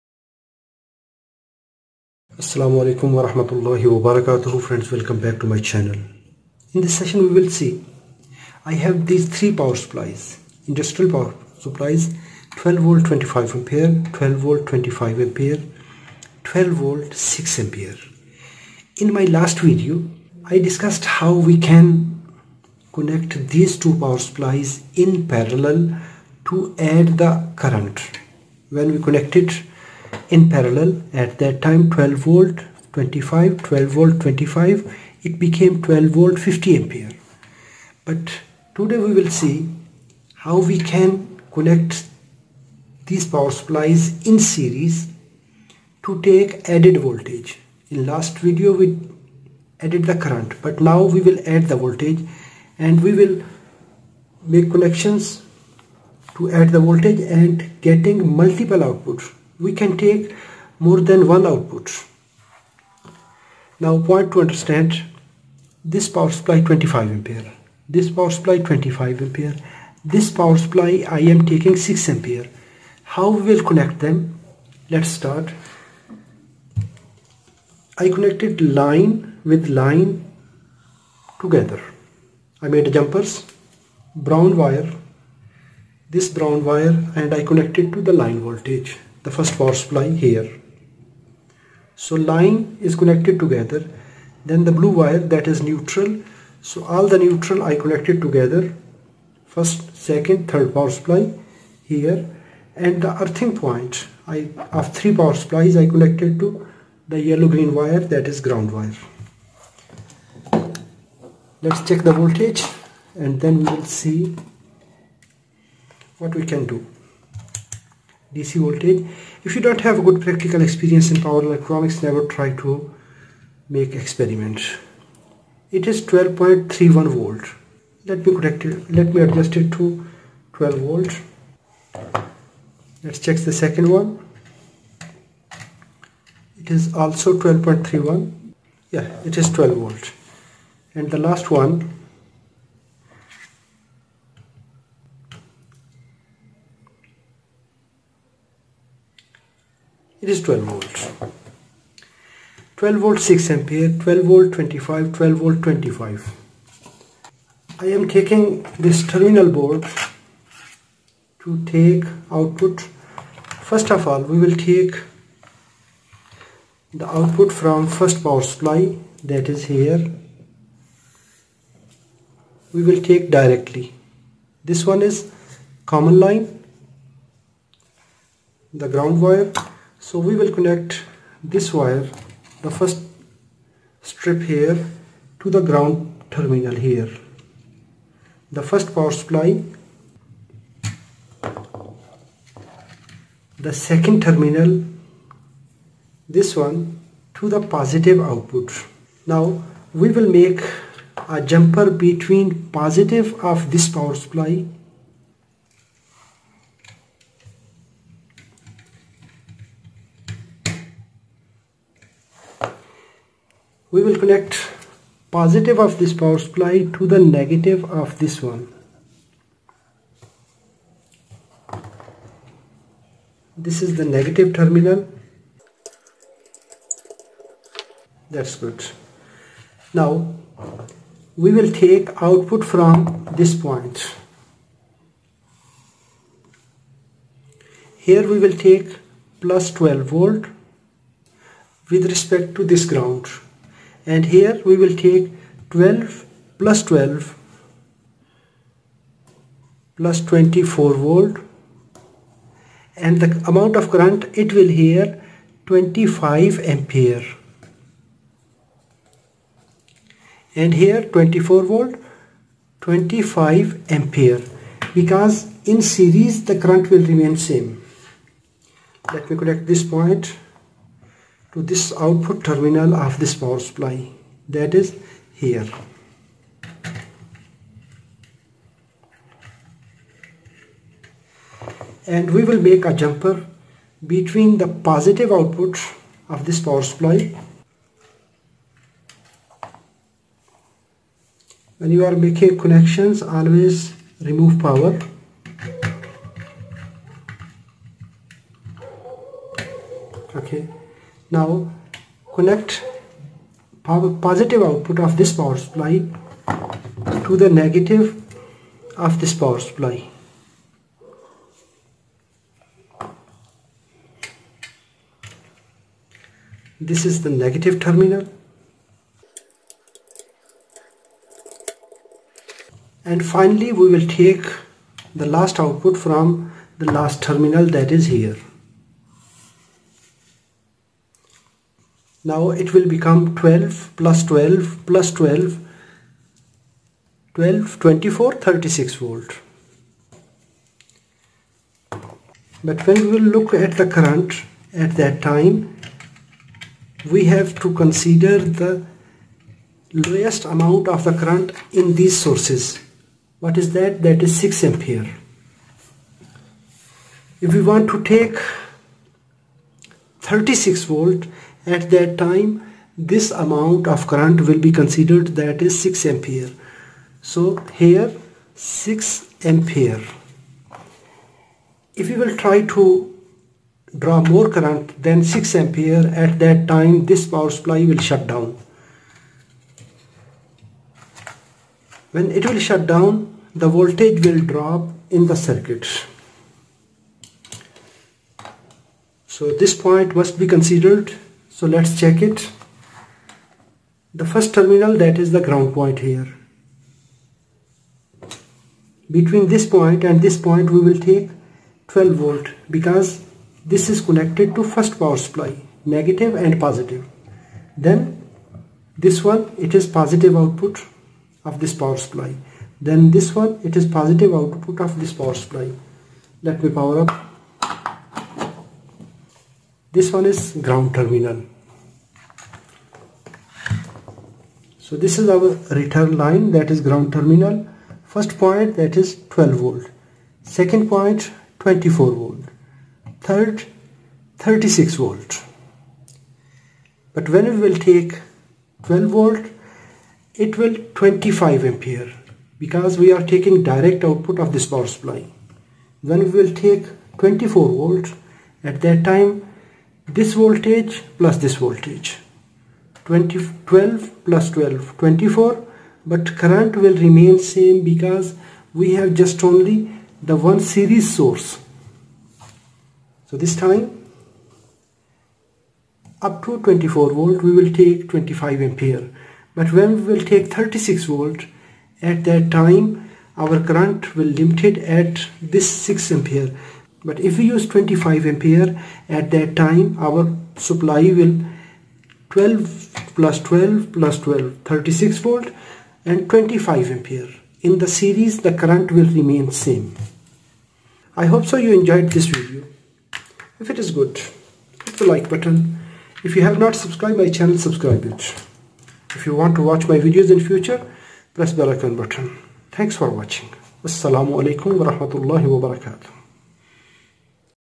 {598} Connect Power Supplies in Series to Get More Voltage / Two or More Power Supplies in Series
in this video i demonstrated how to Connect Power Supplies in Series to Get More Voltage / two or More Power Supplies in Series.  i shared the series connection circuit diagram with practical demonstration.  in this process we connect power supplies in series (head to tail rule) -ve of first power supply will taken as common (return line) and +ve of first power supply will taken as a +ve output voltage,
the same positive output is connected to the -ve of the second power supply -ve terminal. and the positive of send power supply will taken out as second +ve output section. and the same +ve will connected to the -ve terminal of the third power supply and +ve of the third power supply will taken as +ve output. thus all power supplies will come in series
so in series connection the voltage will added and the current will considered  equivalent to the minimum current capability in series